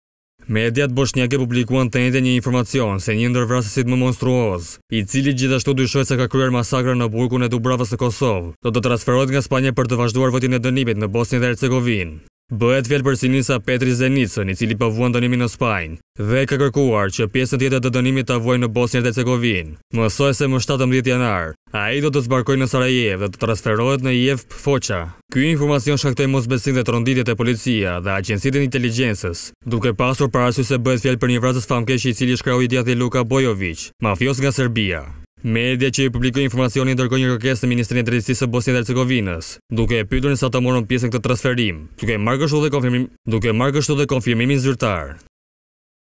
Arrestohet vrasësi i shqiptarëve/ Ekstradohet në Bosnje
Mediat boshnjake publikuan të enjten një informacion, se një ndër vrasësit më monstruoz, i cili gjithashtu dyshohet se ka kryer masakrën në Burgun e Dubravës në Kosovë, do të transferohet nga Spanja për ta vazhduar vuajtjen e dënimit në Bosnje e Hercegovinë.
Bëhet fjalë për Sinisa Petriç Zenicën, i cili po vuan dënimin në Spanjë dhe ka kërkuar që pjesën tjetër të dënimit ta vuajë në Bosnje dhe Hercegovinë. Mësohet se më 17 janar ai do të zbarkojë në Sarajevë dhe do të transferohet në IEVP Foça.

𝐀𝐮𝐭𝐨𝐫 : NTV